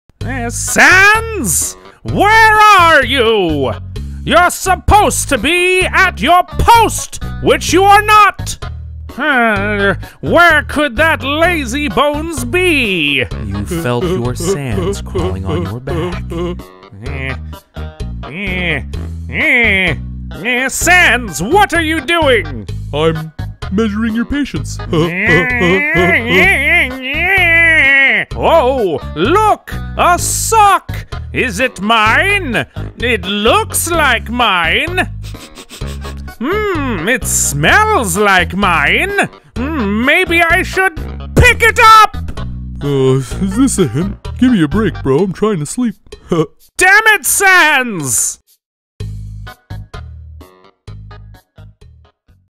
Sans Annoys Papyrus! Undertale Comic Dub!
Sans is with his Brother Papyrus. Things are going well...until Sans decided to mess with his Bro. How will Papyrus react to his SkeleBro's antics? Will he laugh? Or will he get annoyed and angry? Hope yall enjoy this Undertale Comic Dub!